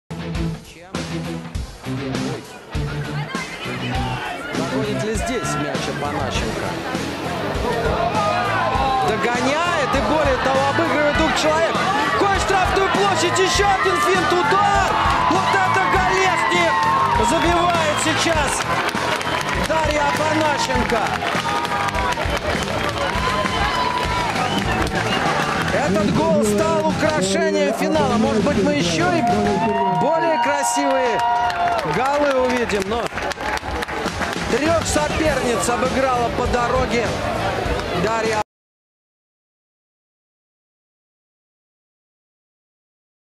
2016, "Звезда-2005"-"Кубаночка", Финал кубка России, Ольга Касаткина
2106, "Звезда-2005"-"Кубаночка", Финал кубка России, Ольга Касаткина. Подарок кинопроекту "Россия с нами не пропадет" из Перми